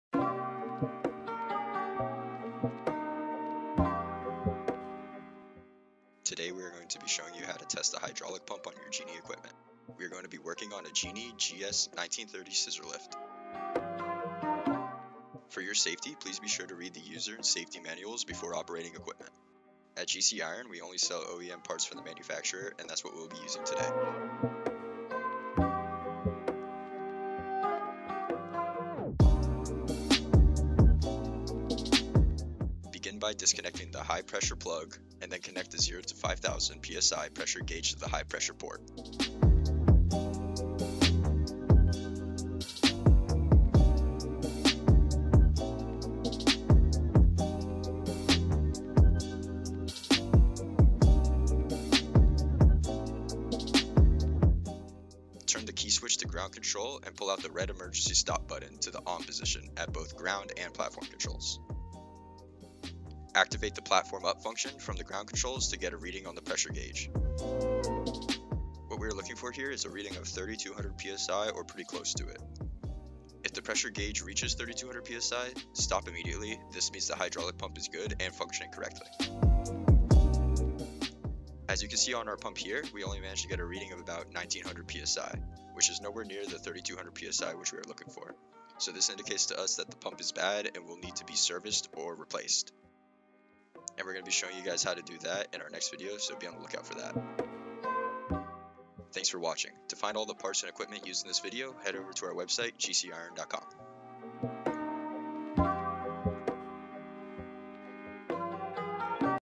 How to Test a Hydraulic Pump
Giving you a look at how to test the hydraulic pump on your Genie equipment

Genuine Genie hydraulic components are designed by Genie engineers to keep your machines running smoothly. At GCIRON.com we offer genuine Genie hydraulic parts for your scissor lifts, boom lifts, and telehandlers